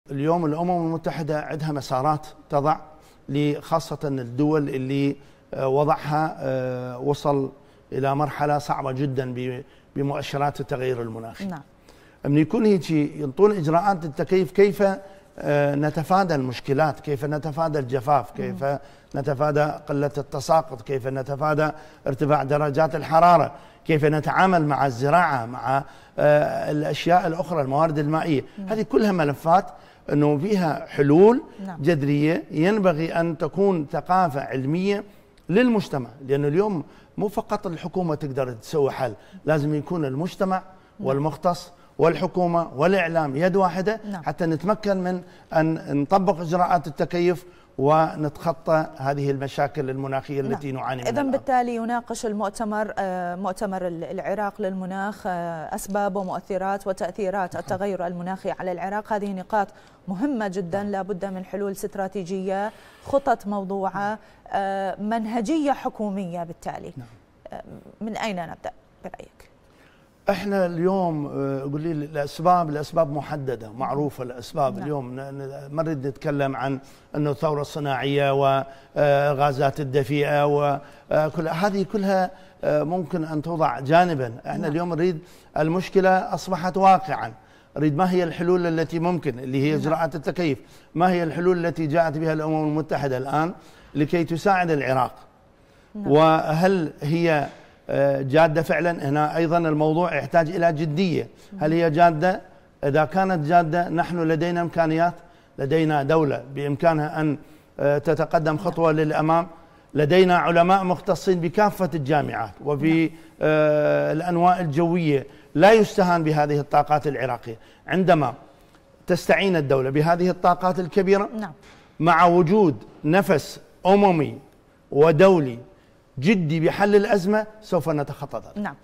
د. سعد عجيل: يجب البدء باجراءات التكيف المناخي من الصفر ويكون المواطن صاحب دور في هذه الاجراءات
هل يستطيع العراق مواجهة التغيرات المناخية؟ في تغطية_خاصة مع نور الهدى الصفار
الضيوف:
د سعد عجيل الدراجي | استاذ الجغرافية الطبيعية
د. نظير عبود فزع | عضو اللجنة العلمية لمؤتمر العراق للمناخ - البصرة
سليم العتابي | معاون مدير عام الهيأه العامة لتشغيل حوض نهر دجلة - واسط